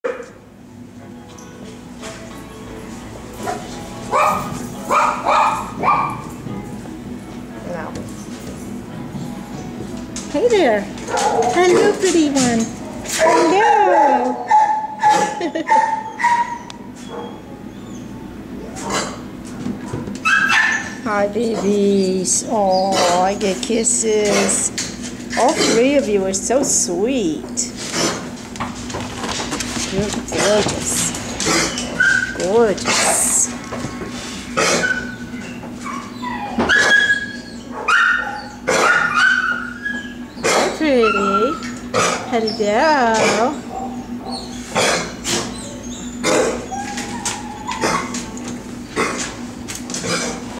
MDAS - November 30, 2016
Three gorgeous dogs, all very friendly and sweet, need adopters now!